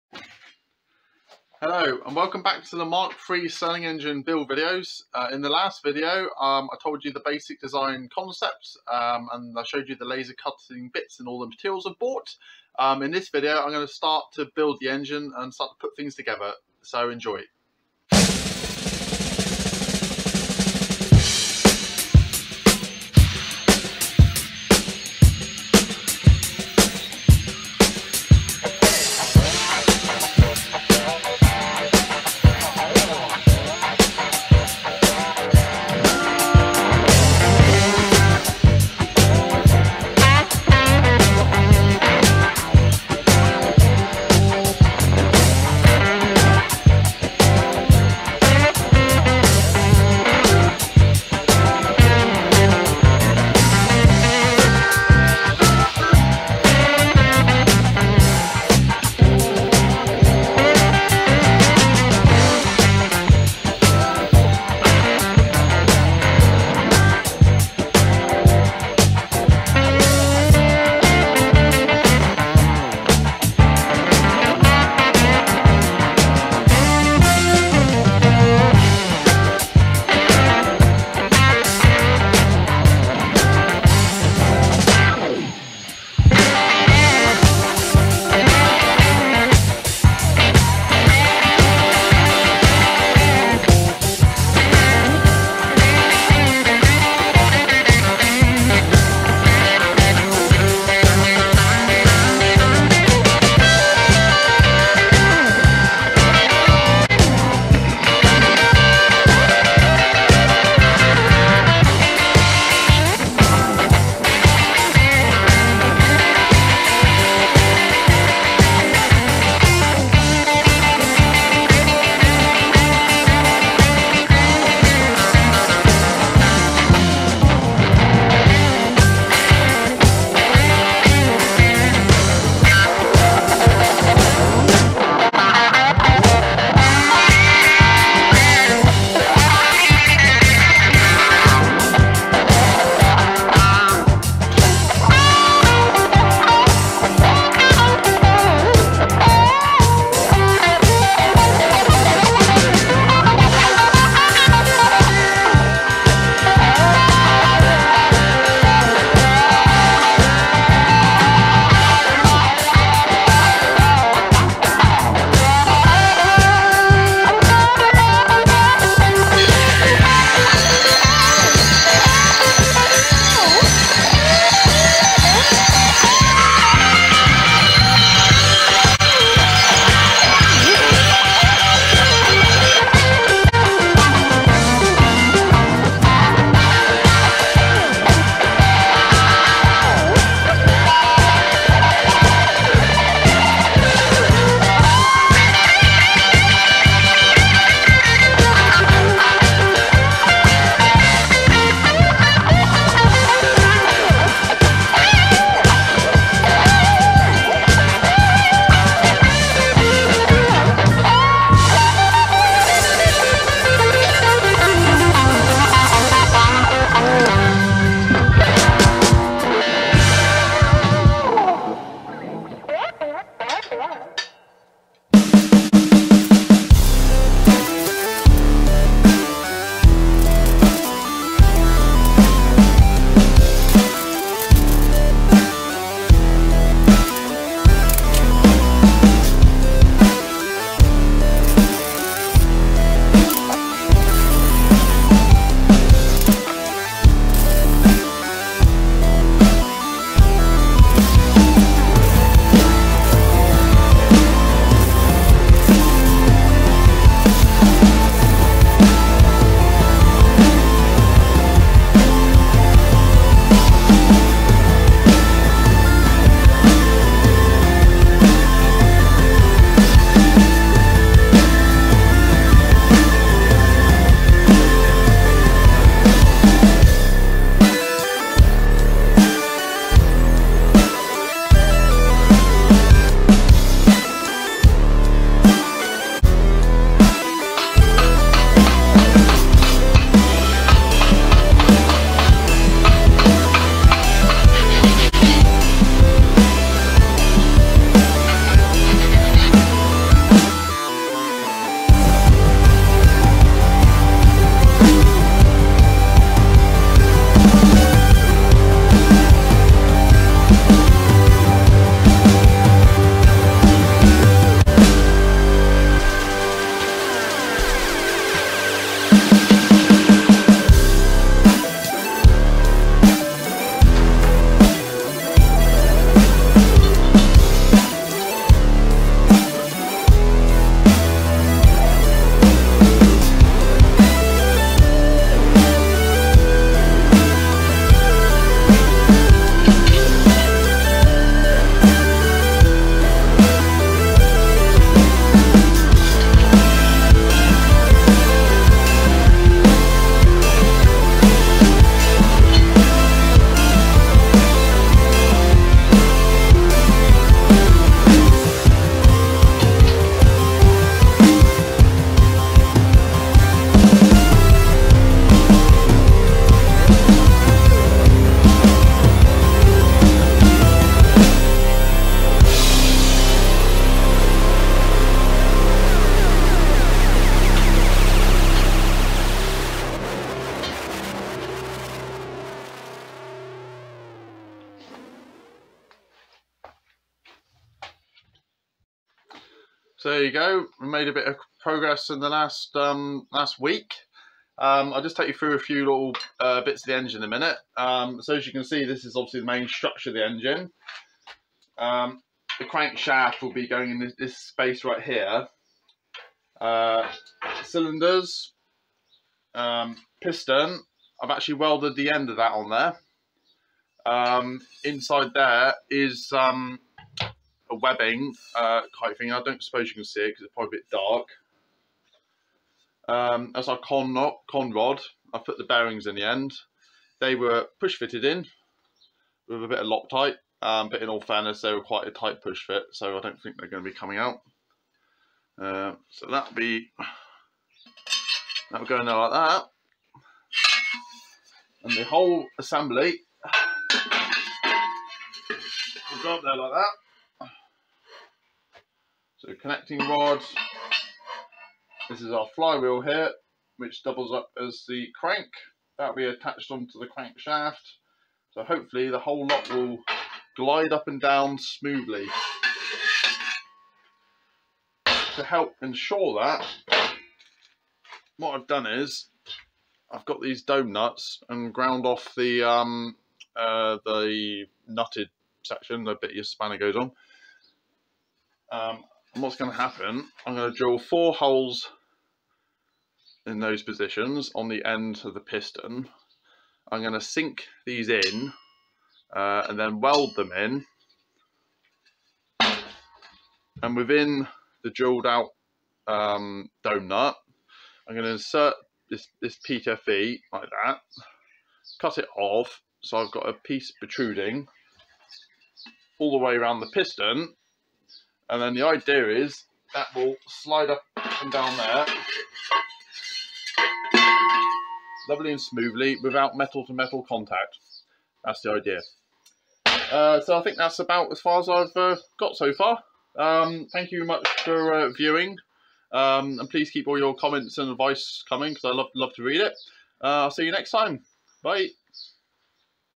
Building a Stirling Engine that is Wood Fired - Part 2 - (Hopefully 500W + )
Join me as I build a DIY stirling engine generator. Will it work?

I want to build a practical stirling engine that runs on wood / biomass. It's got to be home built and its got to generate at least 500w ( hopefully 1kw ! ).

Contents
0:00 Introduction
0:00
0:00
0:00
0:00
0:00 End

So I've designed and built the Mk3 stirling engine and now need to build it

In this video I've assembled the base section and got the cylinders in place ready to weld the other bits on. Also I've pushed the bearings into the connecting rods and welded the end caps on the pistons

Happy with things so far and excited to see how it turns out (and if it works!). In the next video I will weld the firebox together and start to look at putting the heat exchangers together

Please keep your comments / suggestions coming

Cheers